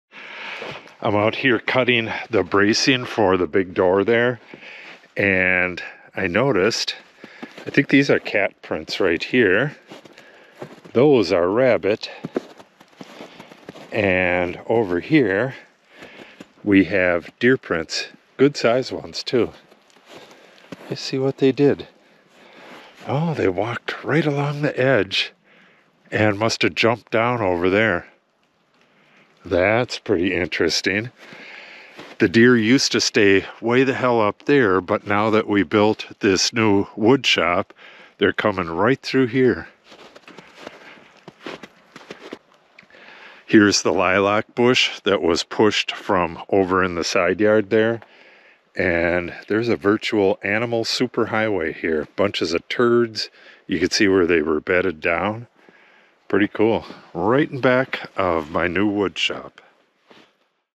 The Deer Are Getting Closer!
The deer are now coming right behind the new woodshop! I was out in the back of the new woodshop today cutting some wood for the overhead door bracing, and noticed deer prints right next to where I was working! It looks like they are now coming about 200' closer to the house than before.